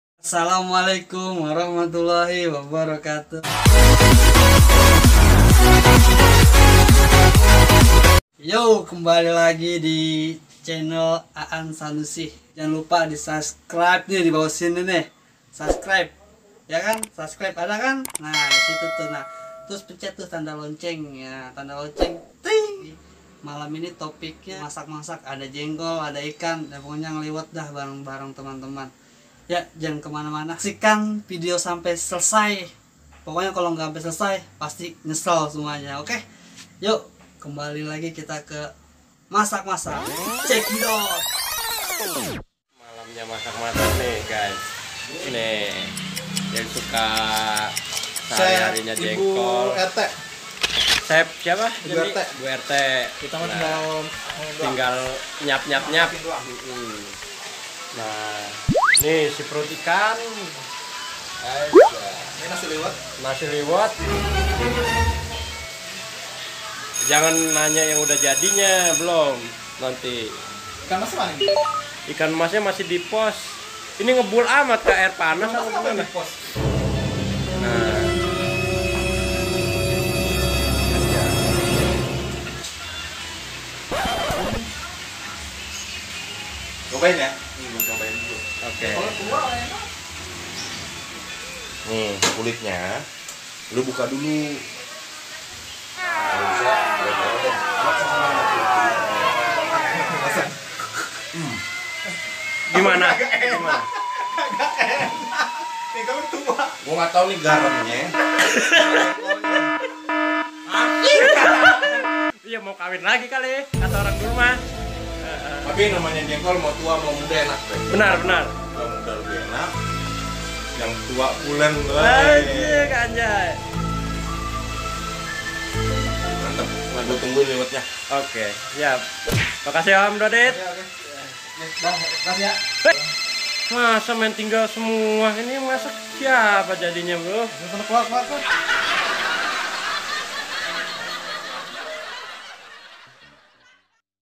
Aan Ngaliweut Bareng part 01
assalamualaikum kawanku semua kali ini aan sanusii channel membuat video acara ngaliwet bareng ,karna kondisi seperti ini kebersamaan selalu ada. ikuti video aan sampai selesai ya...!!!

semoga sehat sellu